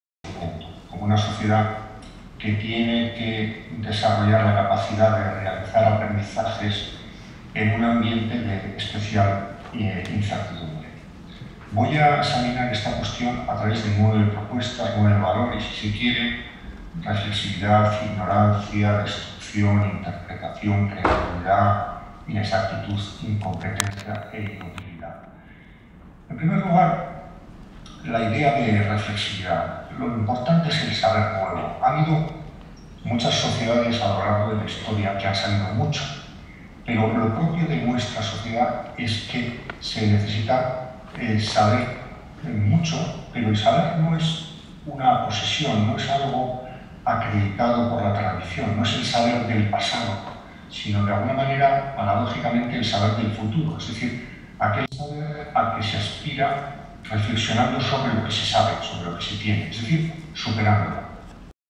III Encontro Educação Oeiras, Daniel Innerarity
III Encontro de Educação de Oeiras, com o tema ‘Educação e Espaço Público’ a 7 de setembro de 2022, no Auditório do Tagus Park.
Conferência de Daniel Innerarity, Instituto de Governanza Democrática, País Basco (excerto)